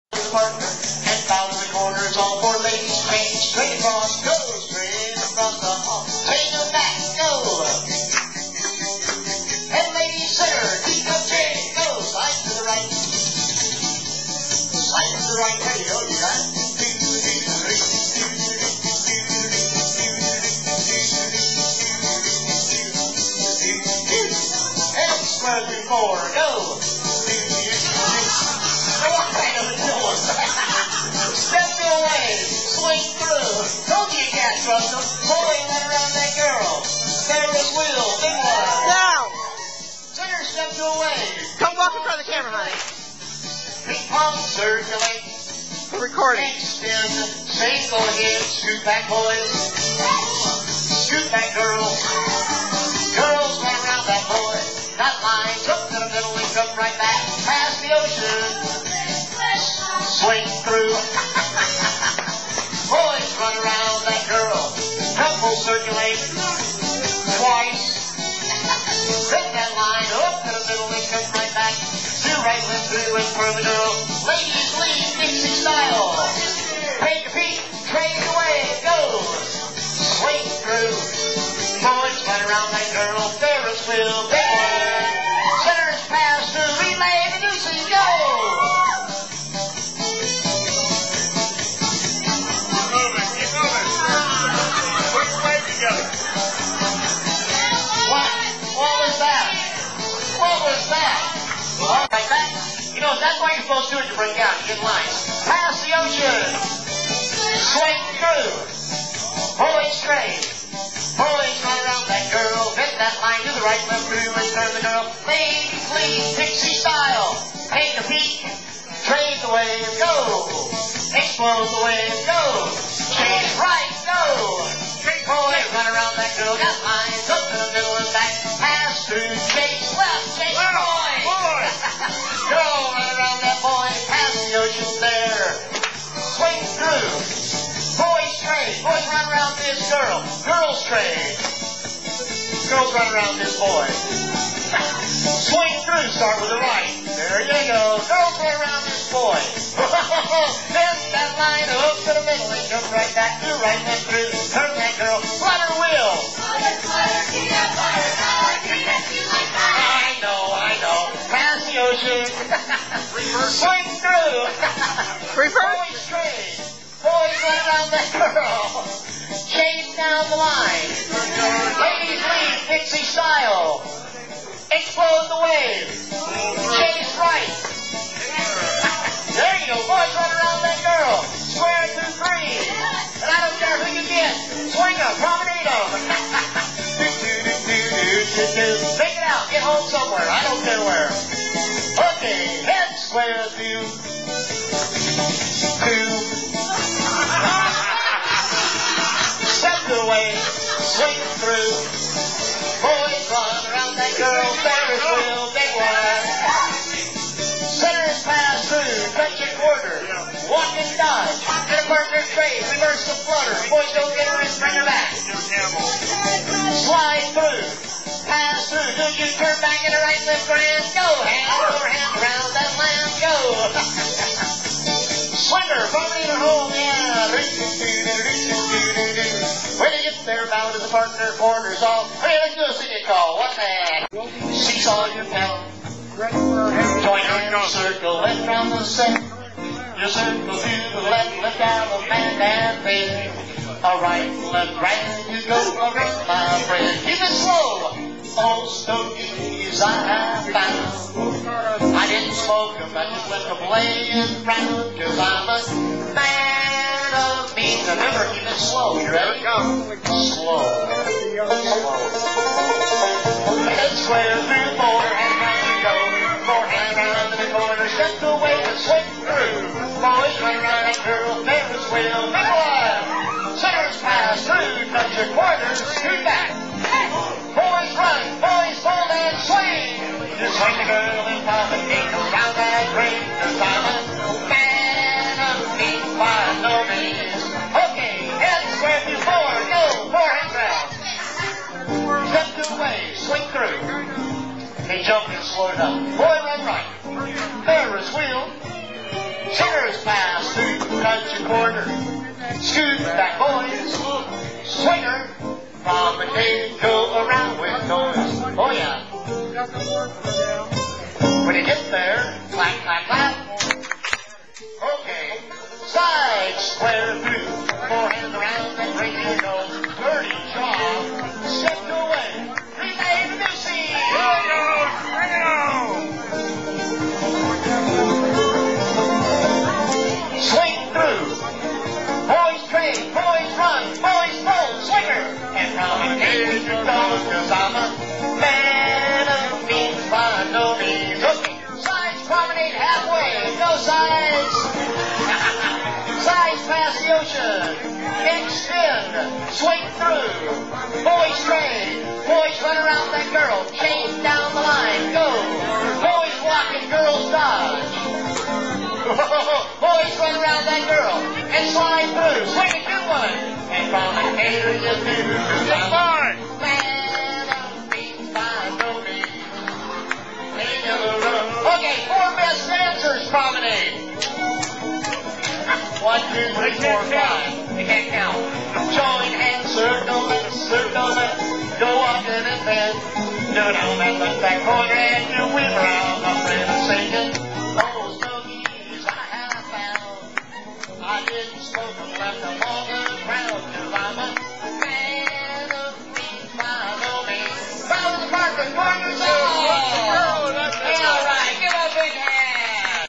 Wagon Train Demonstration
Croakers get together and show the Wagon Train class members what they soon will be learning to do.